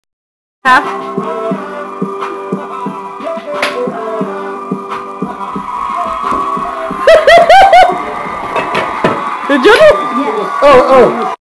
Front room dancin
Corey teachin sissy a dumb ass dance